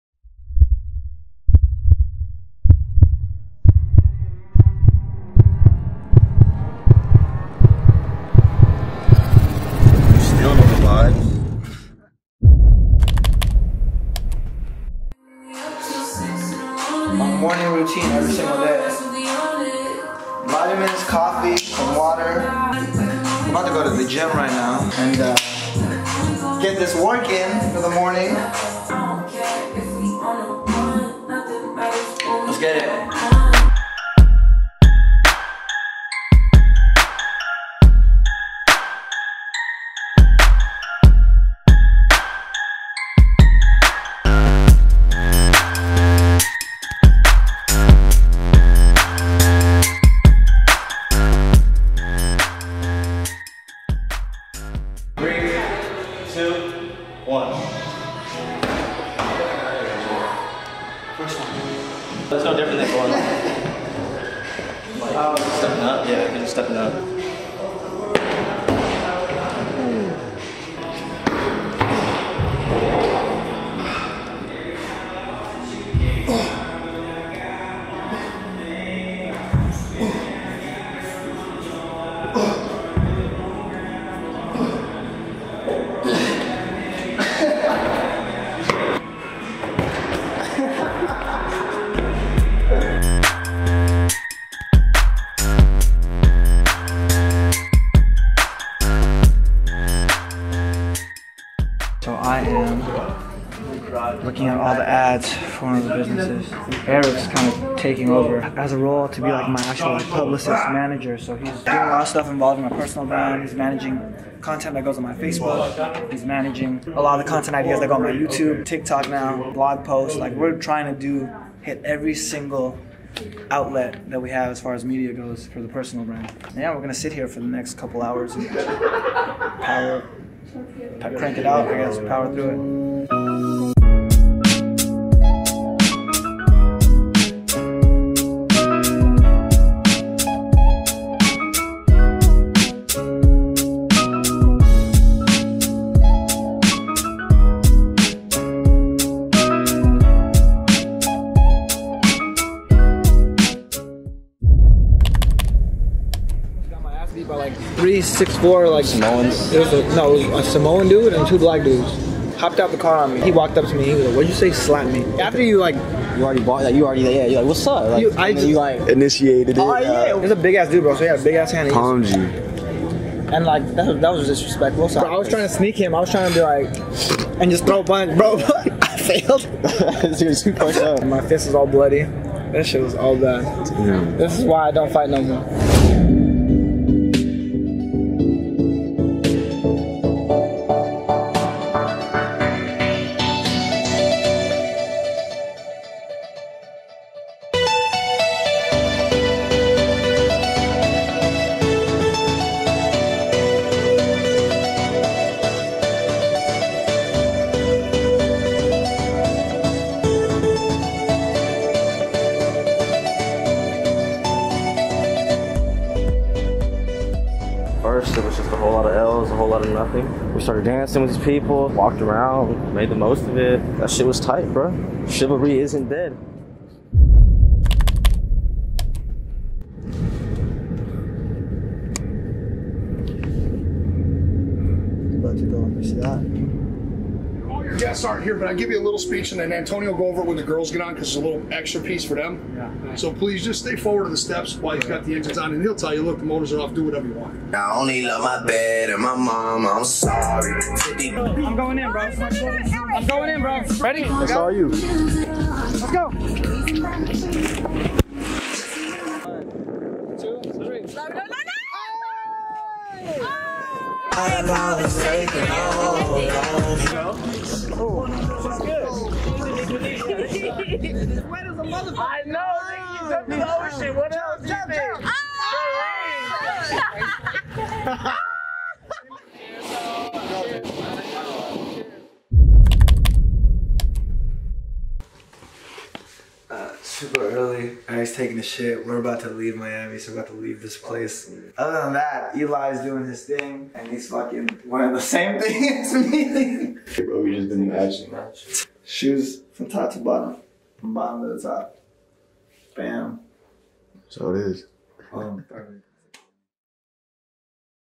Renting A $1,000,000+ Yacht in Miami (VLOG #3) - Rene Lacad
I recently went to Miami for Art Basel and it was one of my more insightful trips. I think creativity is one of the biggest reasons for my success and that's why I'm always on a constant search for inspiration. When I open my mind up to new experiences it allows me to see life from a different perspective. Here's a recap of the entire trip!